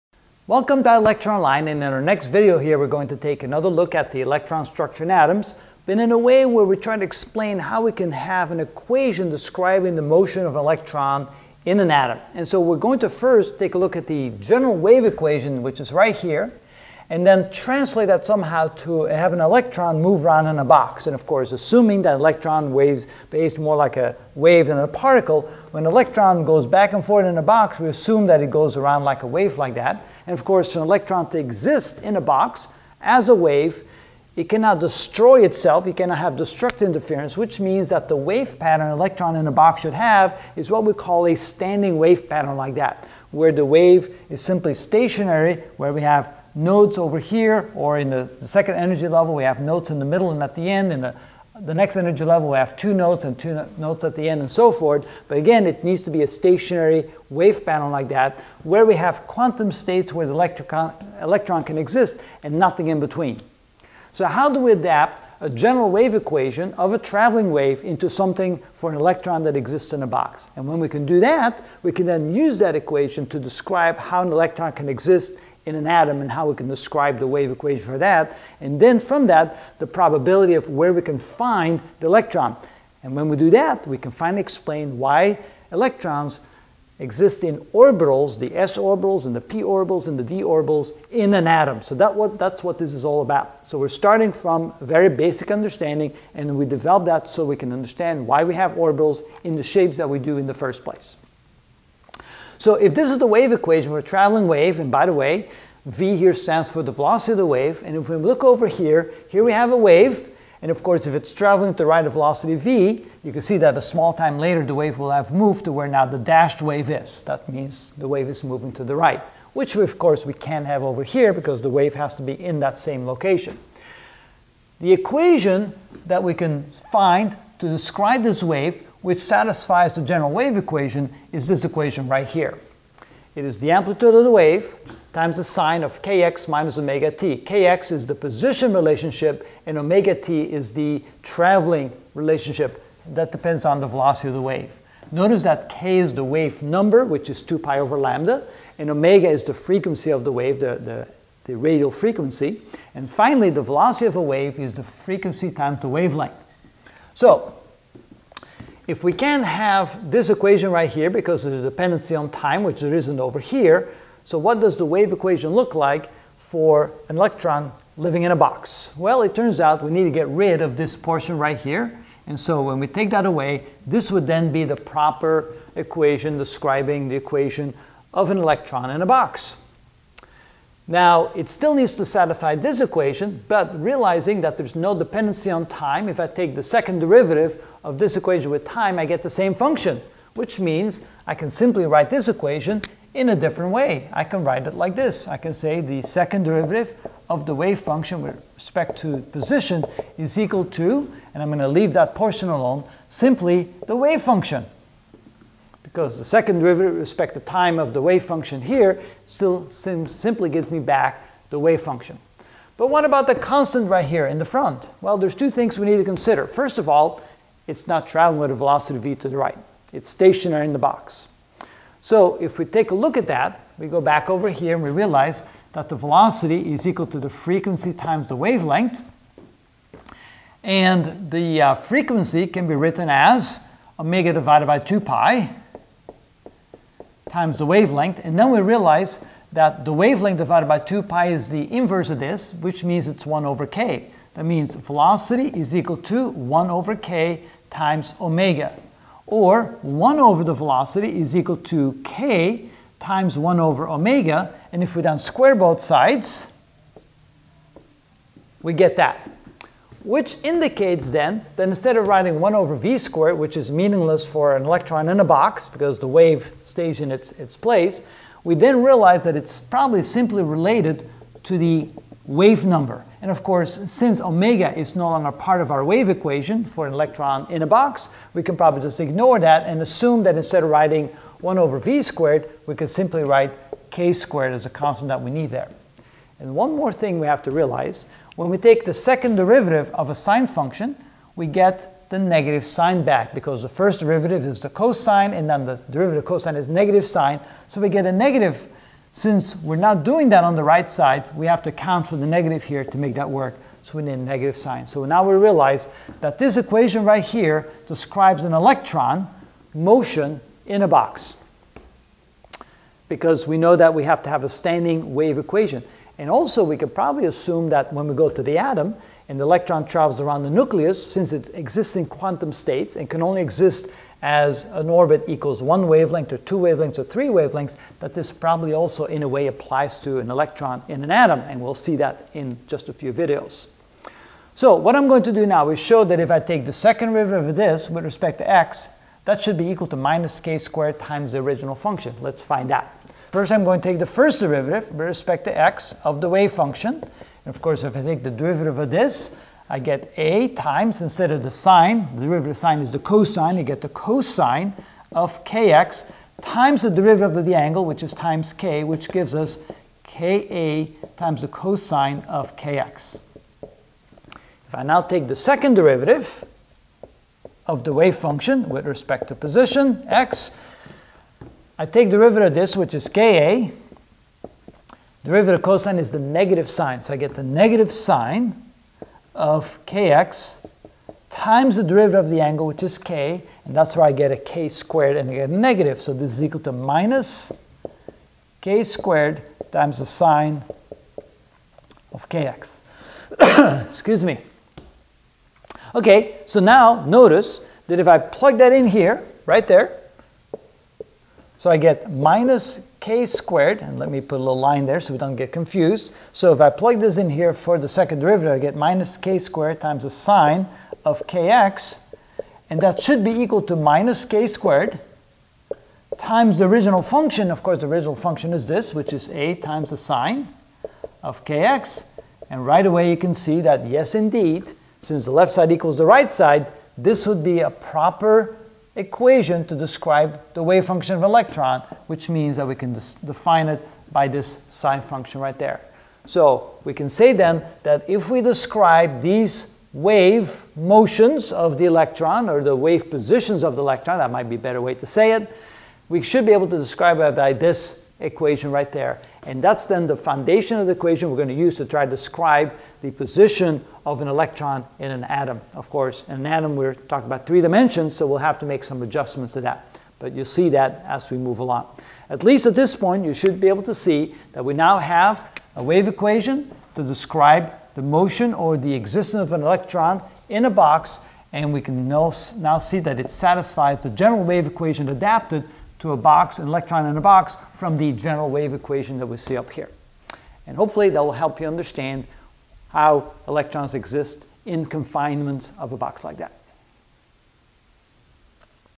Chemistry - Electron Structures in Atoms (17 of 40) The Electron and the Wave Eq: Part 1
In this video I will explain the electron and develops the wave equation for the motion of the electron.